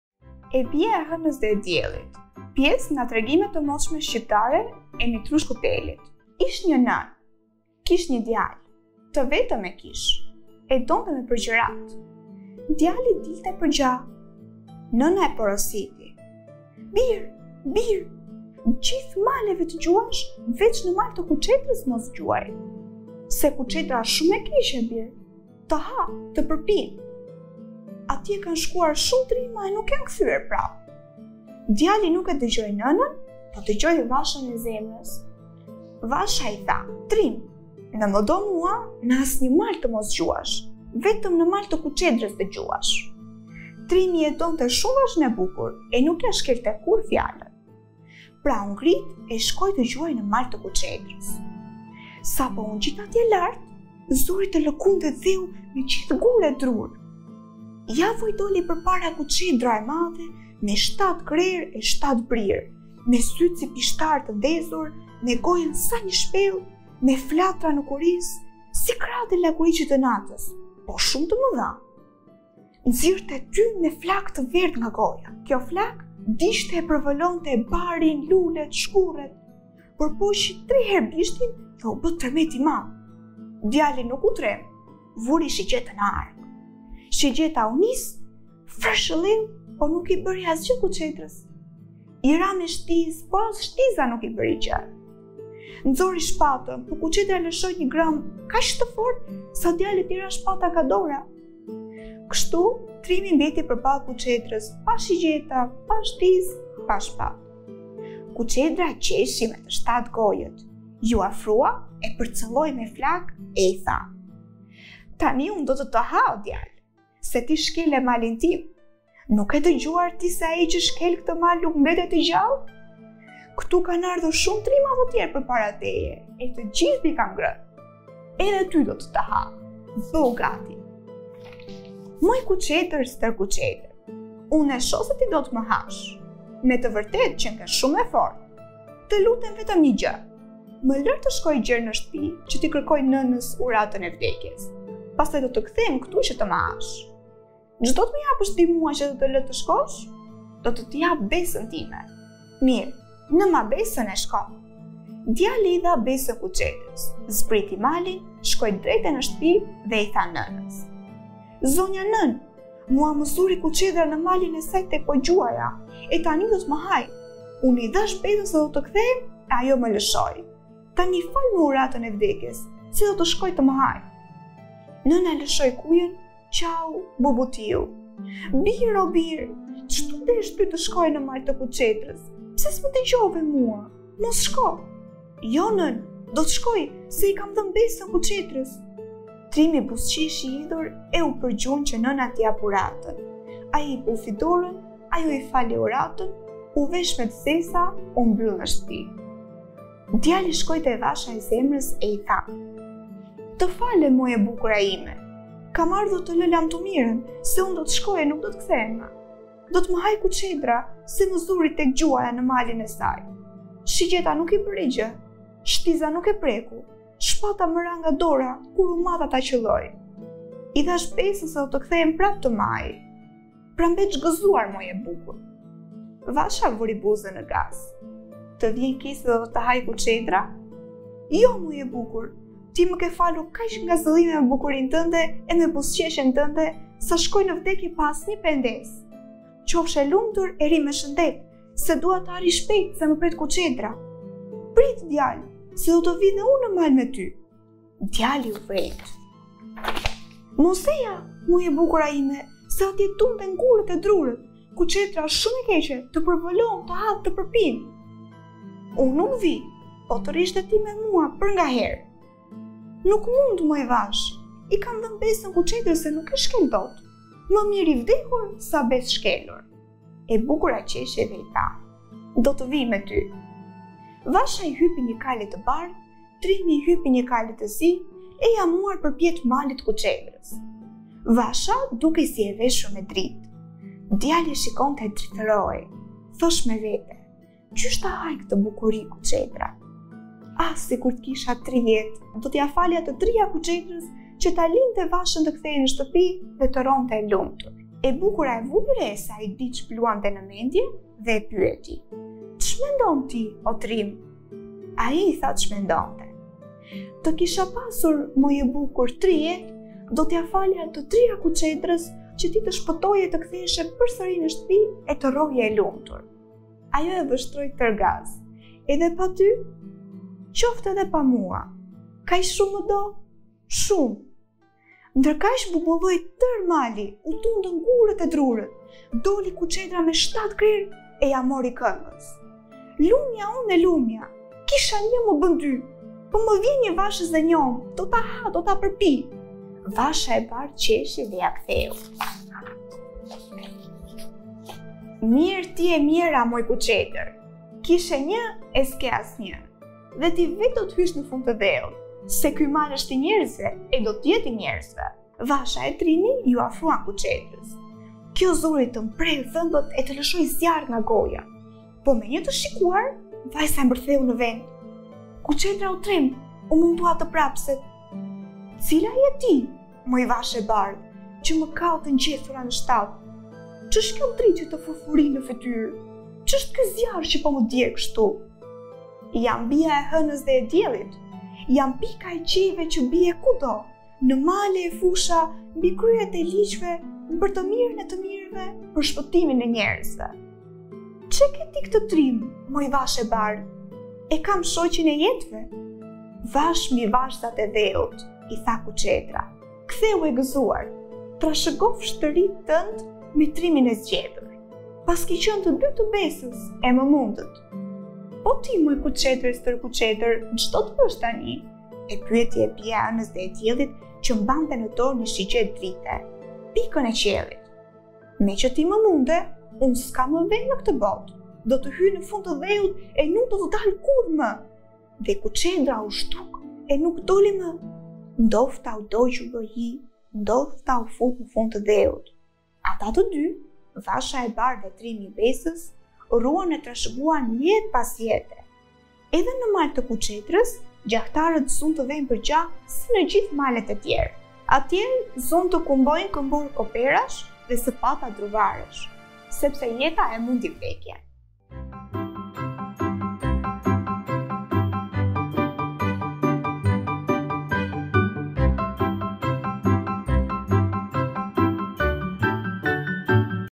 E bija e Henes dhe e Diellit - Pjese nga Tregime te Mocme Shqiptare (e Mitrush...
Ish një nënë, kish një djalë. Të vetëm e kish. E donte me përgjëratë. Djali dilte për gjah. Nëna e porositi: - Bir, bir! Gjithë maleve të gjuash, veç në mal të kuçedrës mos gjuaj. Se kuçedra është shumë e keqe, bir: të ha, të përpin.
Atje kanë shkuar shumë trima e nuk janë kthyer prapë...
Djali nuk e dëgjoi nënën, po e dëgjoi vashën e zemrës. Vasha i tha: - Trim, në më do mua, në asnjë mal të mos gjuash, vetëm në mal të kuçedrës të gjuash! Trimi e donte shumë vashën e bukur e nuk ia shkelte kurrë fjalën. Pra u ngrit e shkoi të gjuajë në mal të kuçedrës. Sapo u ngjit atje lart, zuri të lëkundet dheu me gjithë gurë e drurë. I doli përpara kuçedra e madhe me shtatë krerë e shtatë brirë, me sytë si pishtarë të ndezur, me gojën sa një shpellë, me flatra në kurriz si krahët e lakuriqit të natës, po shumë të mëdha. Nxirrte tym e flakë të verdhë nga goja. Kjo flakë digjte e përvëlonte barin, lulet, shkurret. Përpoqi tri herë bishtin dhe u bë tërmet i madh. Djali nuk u tremb: vuri shigjetën në hark. Shigjeta u nis, fërshëlleu po nuk i bëri asgjë kuçedrës. I ra me shtizë, po as shtiza nuk i bëri gjë. Nxori shpatën, po kuçedra lëshoi një grahmë kaq të fortë sa djalit i ra shpata nga dora.
Kështu, trimi mbeti përballë kuçedrës pa shigjeta, pa shtizë, pa shpatë. Kuçedra qeshi me të shtatë gojët, iu afrua, e përcëlloi me flakë e i tha: - Tani unë do të të ha, o djalë, se ti shkele malin tim. Nuk ke dëgjuar ti se ai që shkel këtë mal nuk mbetet i gjallë? Këtu kanë ardhur shumë trima të tjerë përpara teje e të gjithë i kam ngrënë. Edhe ty do të të ha. Bëhu gati!
- Moj kuçedër stërkuçedër, unë e shoh se ti do të më hash. Me të vërtetë qënke shumë e fortë. Të lutem vetëm një gjë: më ler të shkoj gjer në shtëpi që t'i kërkoj nënës uratën e vdekjes. Pastaj do të kthehem këtu që të më hash. - Ç'do të më japësh ti mua që të të le të shkosh?
- Do të të jap besën time. - Mirë, nëma besën e shko.
Djali i dha besën kuçedrës, zbriti malin, shkoi drejt në shtëpi e i tha nënës:
- Zonja nënë, mua më zuri kuçedra në malin e saj tek po gjuaja e tani do të më hajë. Unë i dhashë besën se do të kthehem e ajo më lëshoi. Tani falme uratën e vdekjes se do të shkoj të më hajë. Nëna lëshoi kujën, qau, bubutiu:
- Bir, o bir! Ç't'u desh ty të shkoje në mal të kuçedrës? Pse s'më dëgjove mua? Mos shko! - Jo, nënë, do të shkoj, se i kam dhënë besën kuçedrës.
Trimi buzëqeshi hidhur e u përgjunj që nëna t'i japë uratën; ai i puthi dorën, ajo i fali uratën, u vesh me të zeza, u mbyll në shtëpi... Djali shkoi tek vasha e zemrës e i tha: Te falem, moj e bukura ime! Kam ardhur të të le lamtumirën se unë do të shkoj e nuk do të kthehem më. Do të më hajë kuçedra se më zuri tek gjuaja në malin e saj. Shigjeta nuk i bëri gjë, shtiza nuk e preku, shpata më ra nga dora kur u mata ta qëlloj. I dhashë besën se do të kthehem prapë të më hajë. Pra, mbeç gëzuar, moj e bukur!
Vasha vuri buzën në gaz. Të vjen keq se do të të hajë kuçedra? Jo, moj e bukur. Ti më ke falur kaq ngazëllime me bukurinë tënde e me buzëqeshjen tënde, sa shkoj në vdekje pa asnjë pendesë. Qofsh e lumtur e rri me  hëndet, se dua të arrij shpejt, më pret kuçedra. Prit, djalë, se do të vij edhe unë në mal me ty. Djali u vrenjt.
- Mos eja, moj e bukura ime, se atje tunden gurët e drurët; kuçedra është shumë e keqe: të përvëlon, të ha, të përpin. Unë nuk vij, po të rrish edhe ti me mua, përngaherë. Nuk mund, moj vashë; i kam dhënë besën kuçedrës e nuk e shkel dot... Më mirë i vdekur se sa besëshkelur.
E bukura qeshi e i tha:
- Do të vij me ty.
Vasha i hypi një kali të bardhë, trimi i hypi një kali të zi e ia muar përpjetë malit të kuçedrës. Vasha dukej si e veshur me dritë. Djali e shikonte e drithërohej. Thosh me vete: "Qysh ta hajë këtë bukuri kuçedra?
Ah, sikur të kisha tri jetë... Do t'ia falja të tria kuçedrës që ta linte vashën të
kthehej në shtëpi dhe të rronte e lumtur”.
E bukura vuri re se ai dic bluante në mendje dhe e pyeti: Ç'mendon ti, o trim? Ai i tha ç'mendonte: Të kisha pasur, moj e bukur, tri jetë, do t'ia...

🗨 Mirëpresim komentet dhe pyetjet tuaja më poshtë 😊